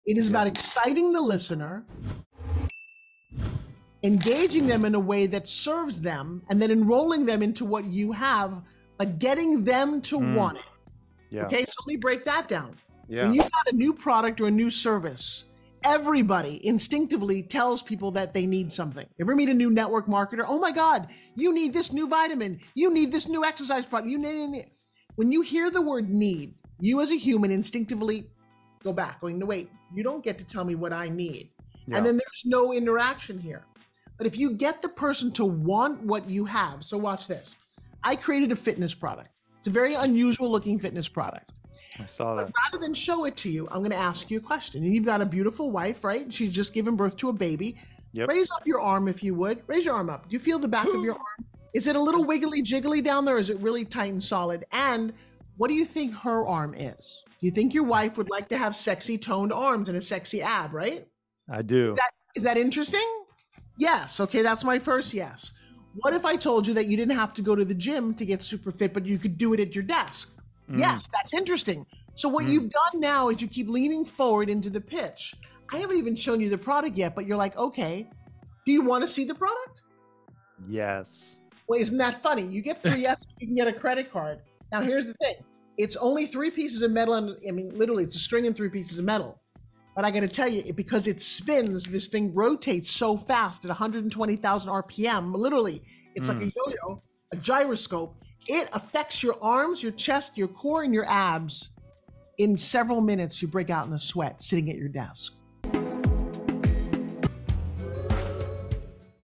Make Your Audience Crave Your Offer By Doing This..
Want to learn the art of pitching?

In this latest episode of the Happy Hustle Podcast, I had the pleasure of chatting with Dr. Forbes Riley, a pioneer and industry leader in infomercials and Home Shopping TV.